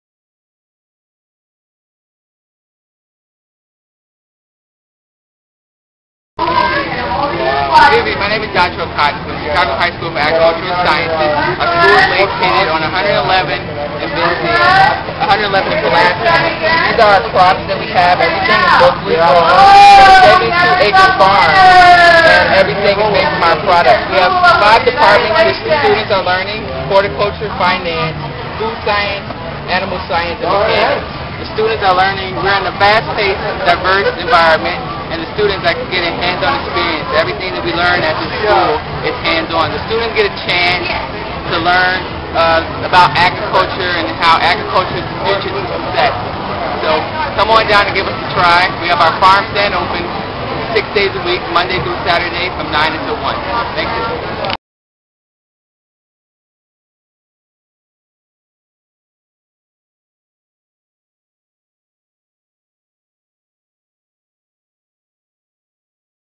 Chicago High School for Agricultural Sciences
Chicago High School for Agricultural Sciences booth at the Eli's Cheesecake festival - September 20, 2008